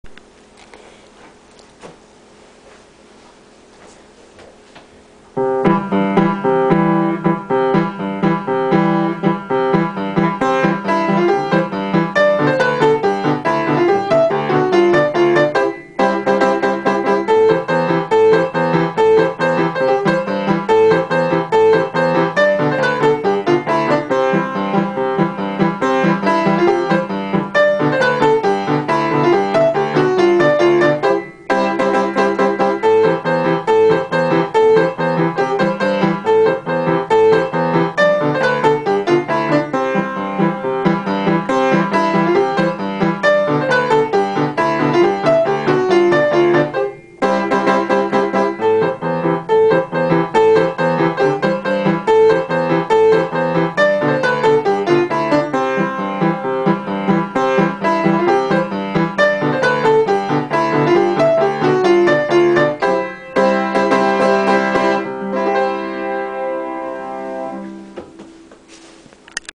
HomeMade Song :)
My dad wrote this song when he was bored, kinda of a funny song, i like it, maybe u can dance with it lol :) enjoy